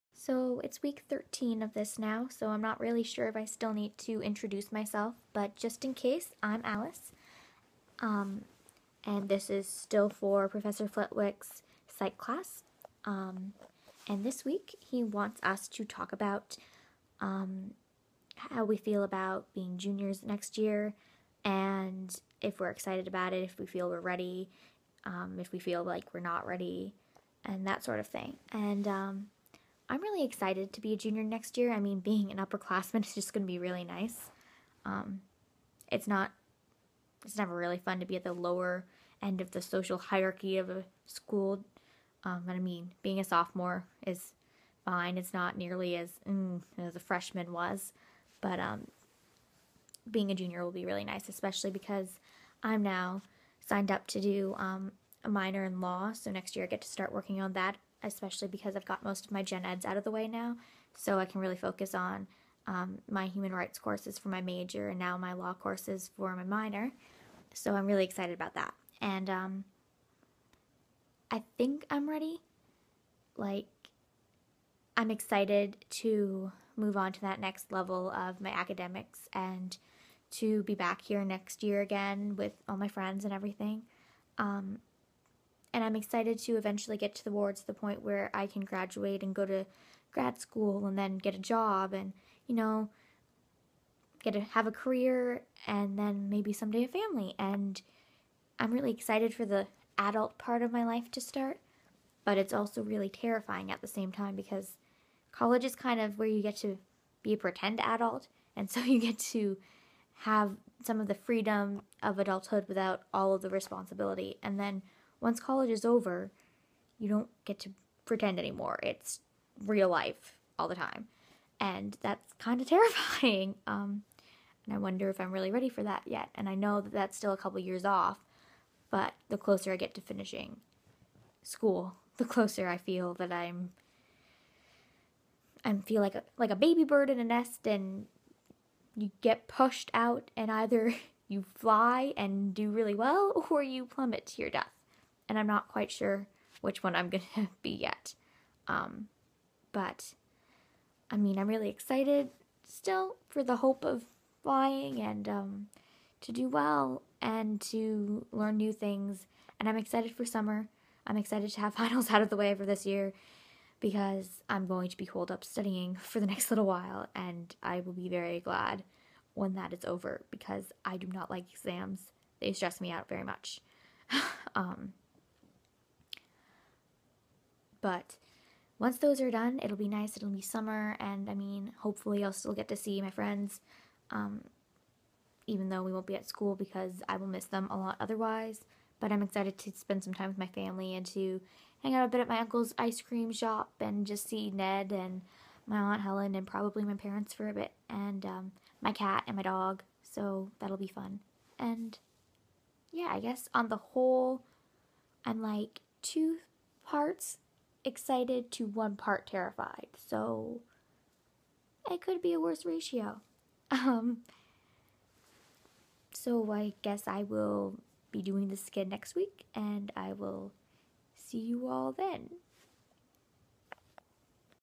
Alice Fortescue Vlog #12: Ready or Not
This week's prompt: if we're ready to be upperclassmen next year. The answer is yes...and no.                                                          -follow Alice on Twitter @ali_fortescue                    Part of the Maraudera Vlog Project (themaraudersvlog.tumblr.com).                             Alice is played by Grace.